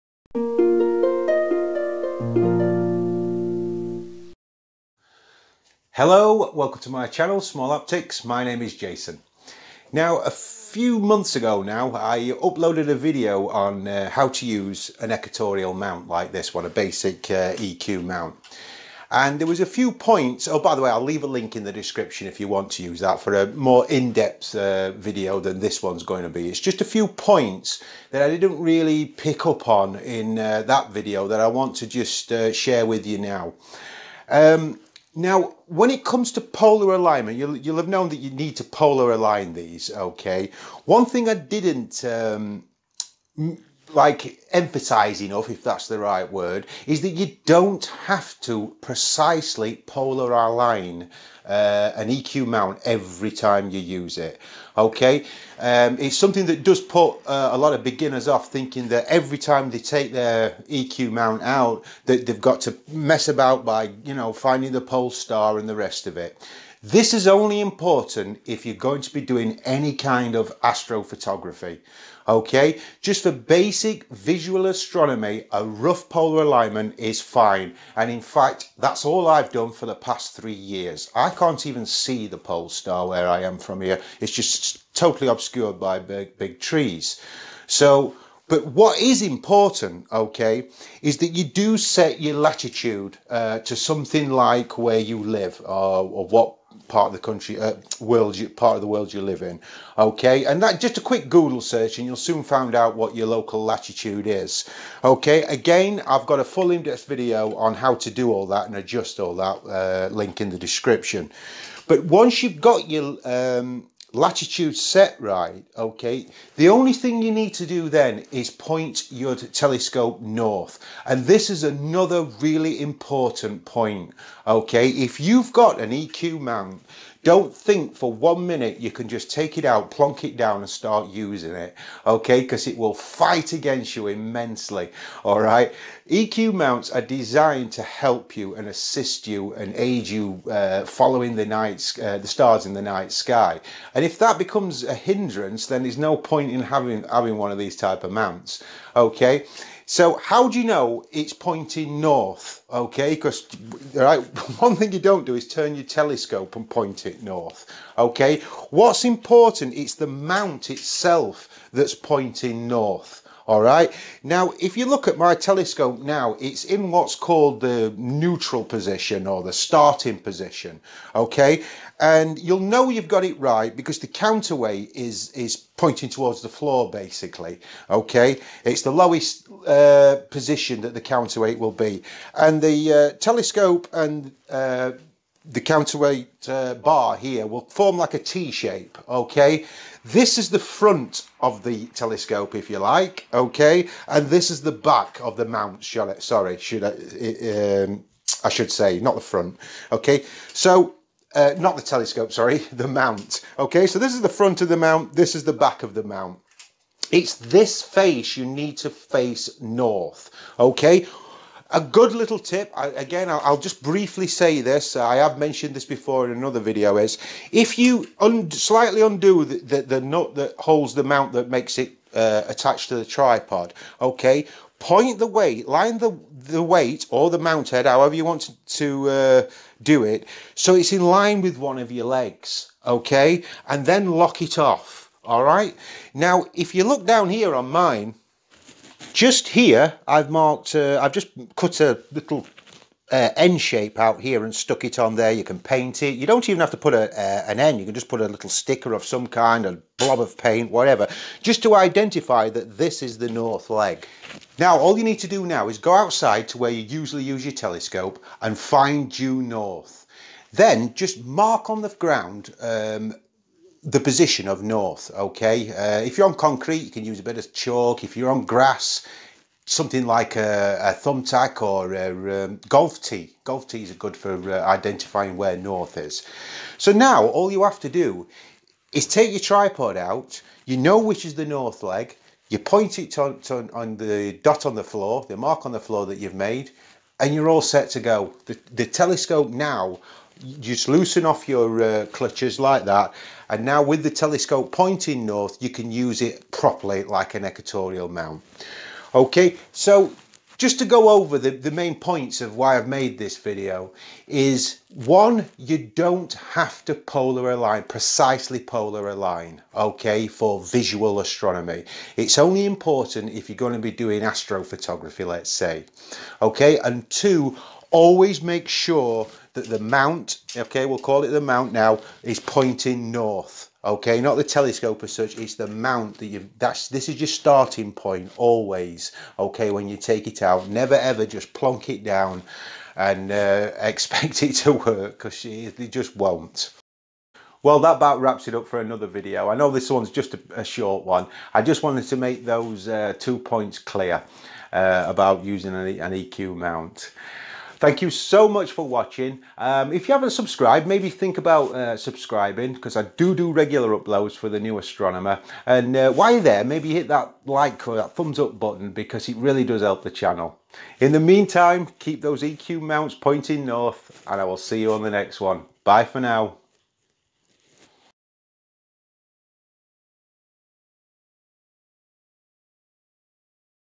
Don't Fight Your EQ Mount
Not using a EQ mount the proper way will only lead to frustration...It will just FIGHT YOU!
In this video I talk about two things that will not only help you to use the mount correctly

but also save you a lot of time...

new astronomer. Take care, clear skies.